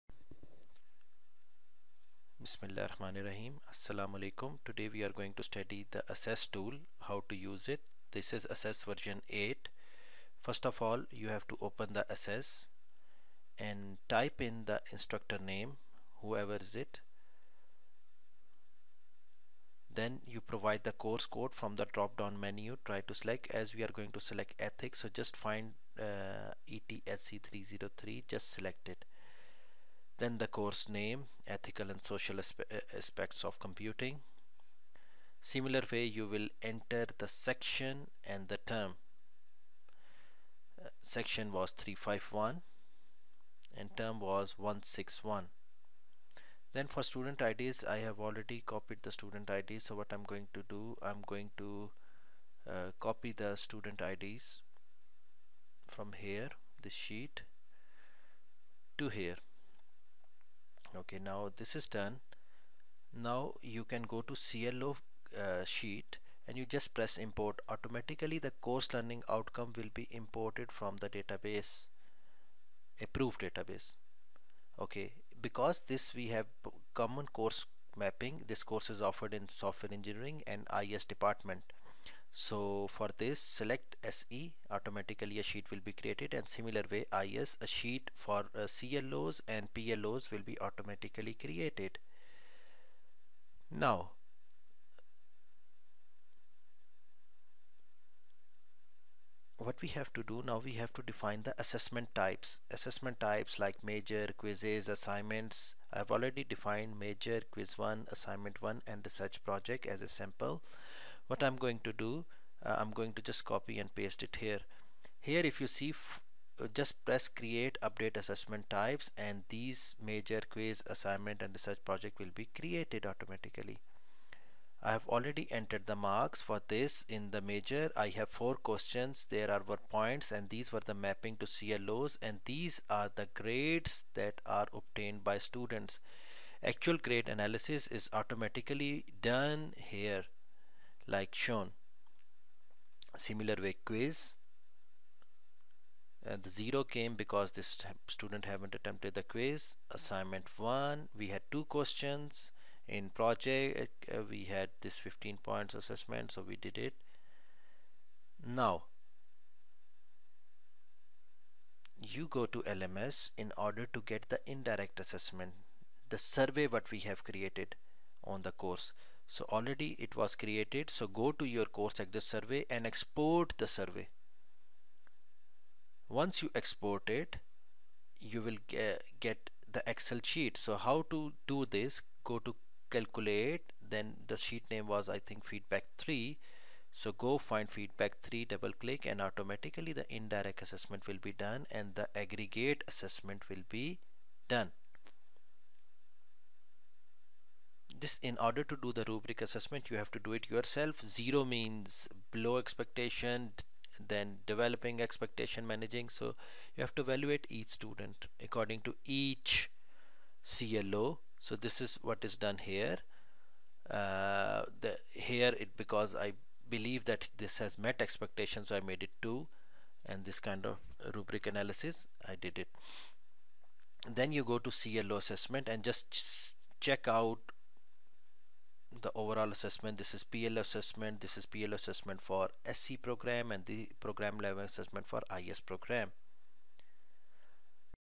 How to Use Assess Tool
This is short video explaining how to use Assess Tool